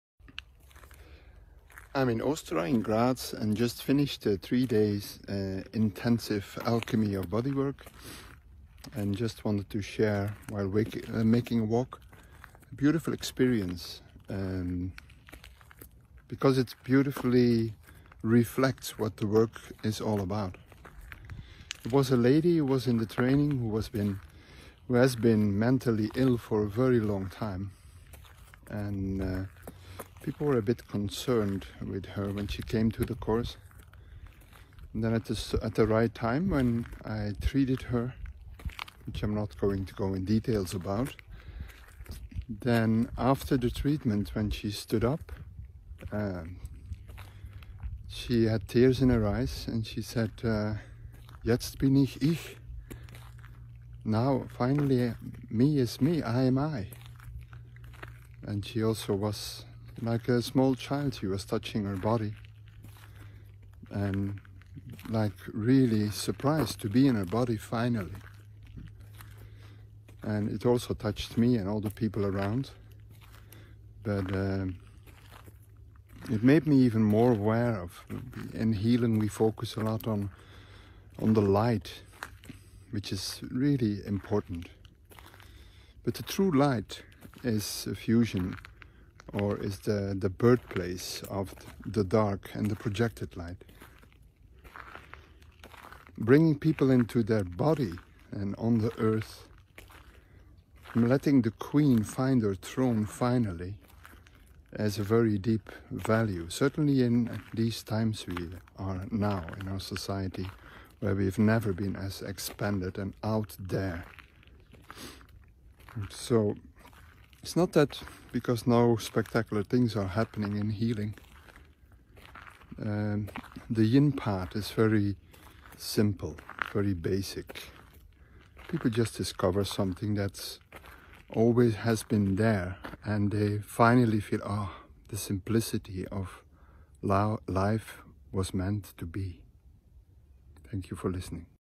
Dirk oellibrandt shares an experience
I want to share this beautiful experience I had during the Alchemy of Bodywork course in Graz. I treated a lady during class who was suffering since a long time from a mental illness